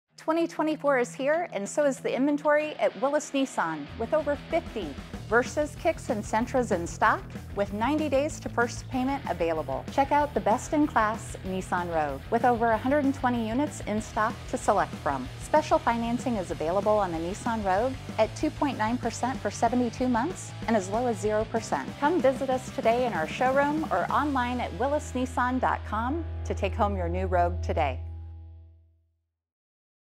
Get 2.9% APR on Nissan Rogues
2024 is here, and so is the inventory at Willis Nissan. We have over 50 Versas, Kicks, and Sentras in stock, all with no payment due for 90 days. With over 120 best-in-class Rogues, we have the perfect one for you to choose from. With financing starting at 0% up to 2.9% for 72 months, now is a great time to buy. So stop in the showroom or visit us online at willisnissan.com.